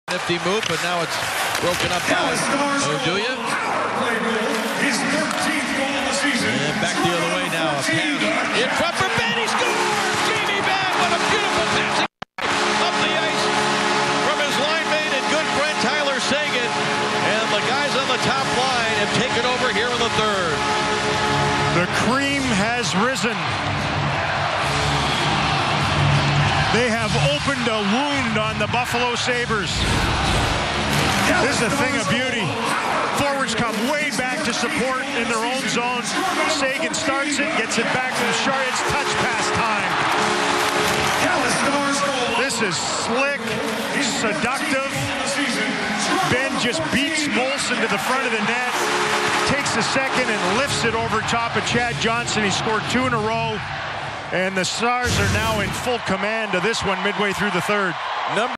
Jamie Benn Scores on Nice Passing Play vs Buffalo
Jamie Benn Goal Scores on Nice Passing Play vs Buffalo Sabres Dallas Stars